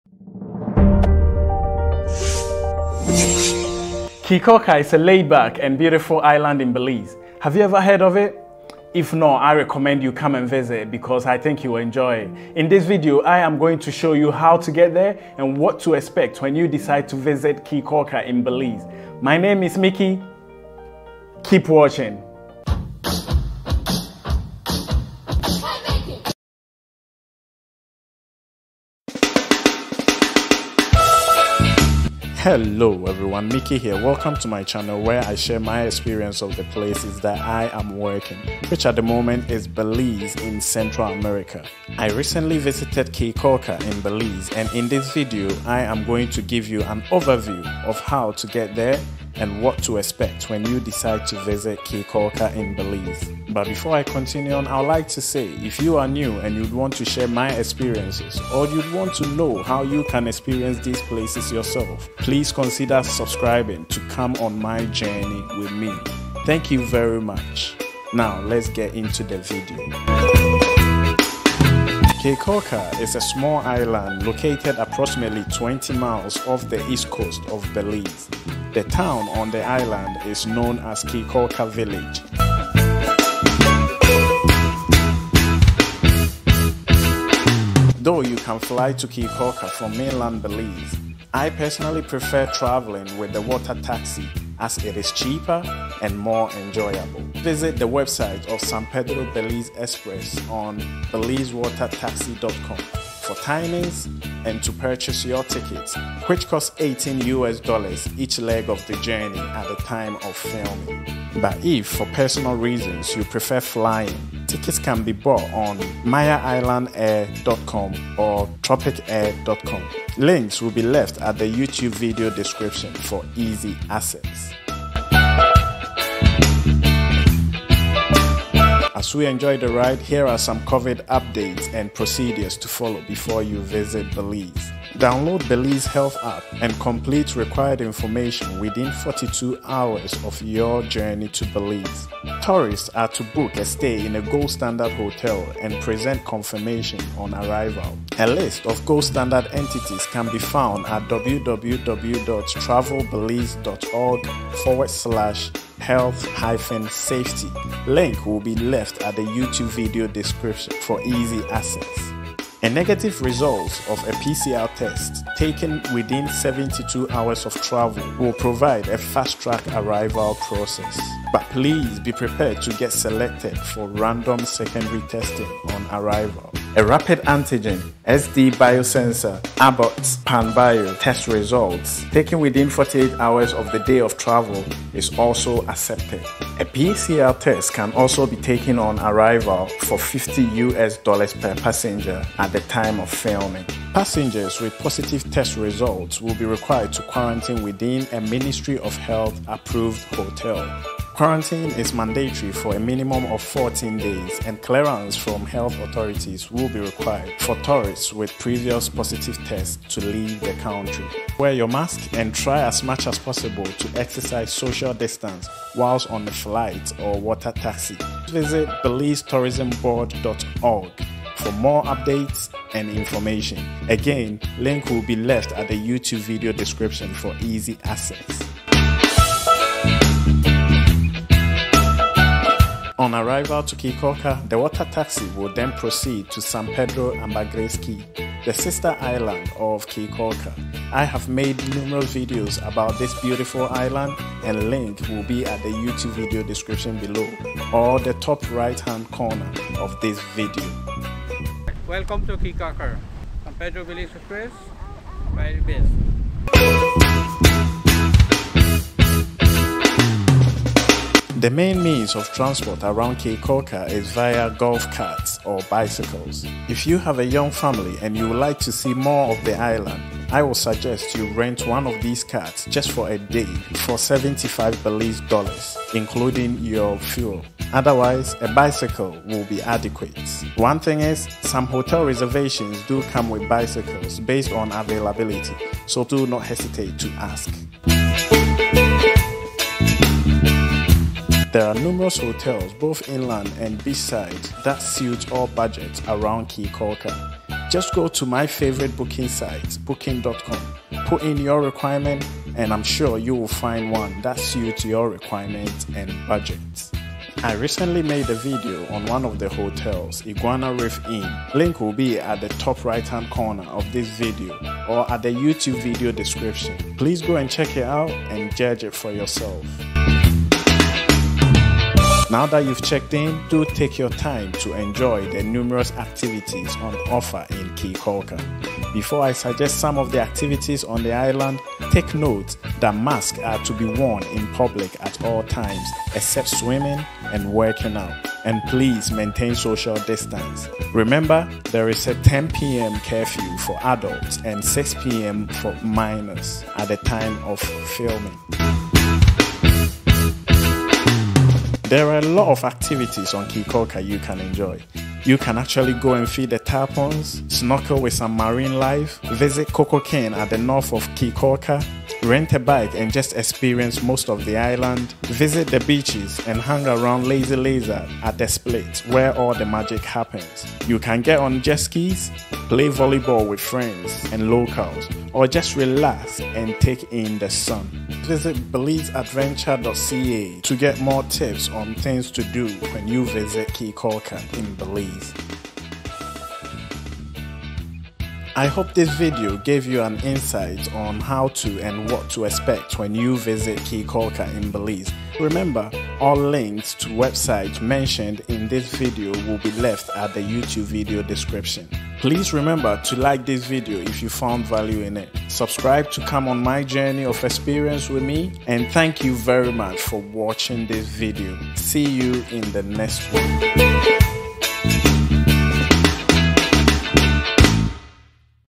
Watch this before you visit Caye Caulker, the sister island of Ambergris Caye in Belize.
Caye Caulker is a laid back and beautiful island in Belize. Have you ever heard of it? If not I will recommend you visit because I think you will enjoy it.

In this video, I am going to show you how to get there and what to expect when you decide to visit Caye Caulker in Belize.

My name is Mickey, keep watching!!!

Laptop for editing
Memory card